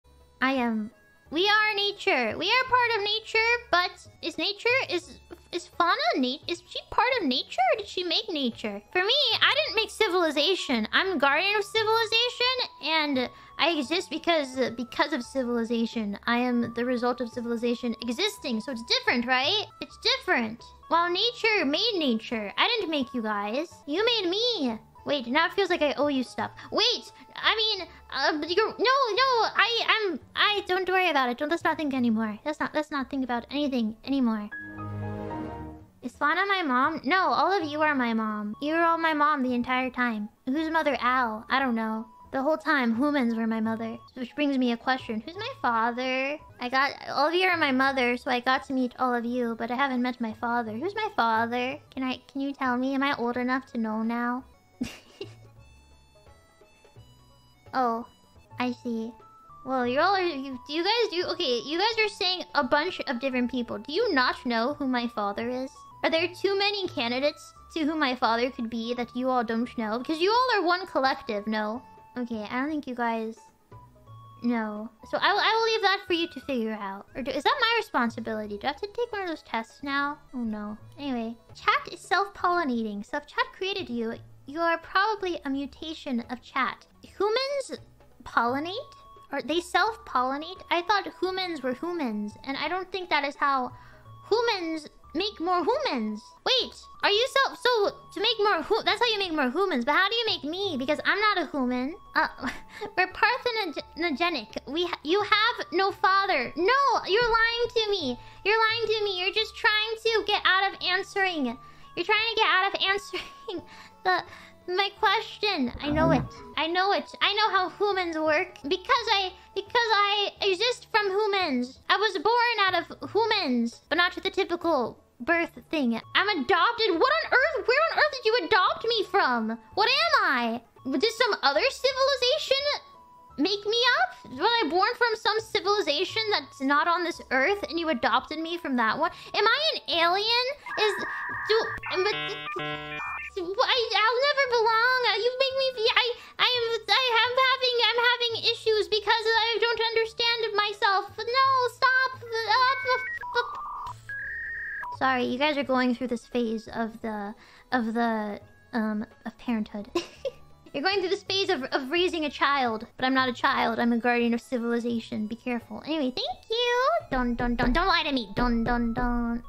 Mumei Learns That Hoomans Are Her Mother... And She Was Adopted【hololive】
Hi, everyone!

Okay guys, how do we tell her?

Take care, and have an excellent day, everyone!